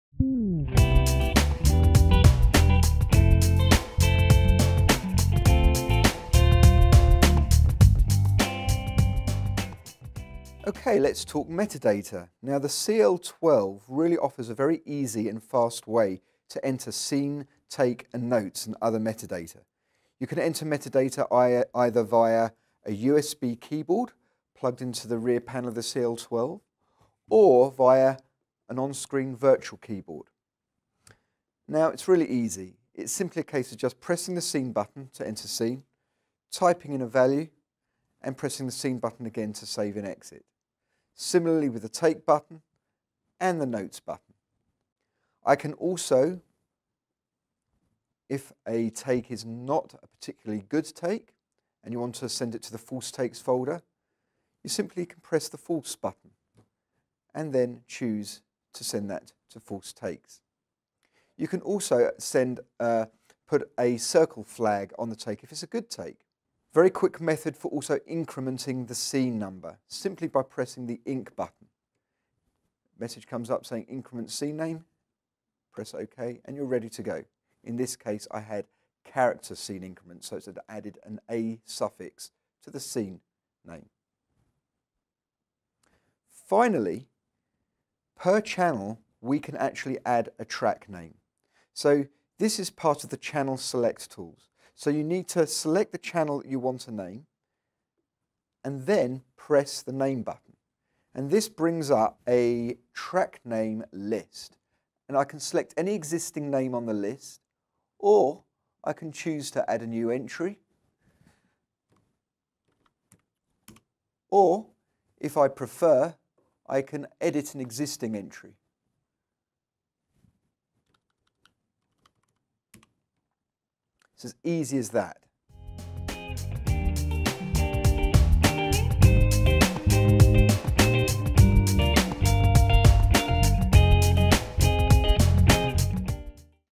Chapter 6: CL-12 Metadata
This is the sixth installment in a series of videos on Sound Devices CL-12 linear fader controller. In Chapter 6, Paul Isaacs, director product management & design, talks about metadata entry with the CL-12.